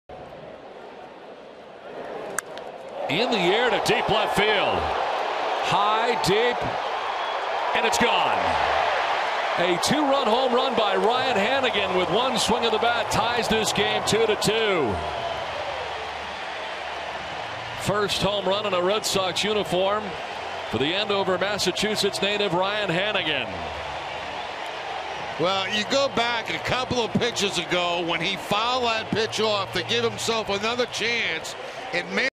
BAL@BOS: Hanigan launches two-run shot to tie game
4/17/15: Ryan Hanigan ties the game with a long two-run home run over the Green Monster in the bottom of the 5th inning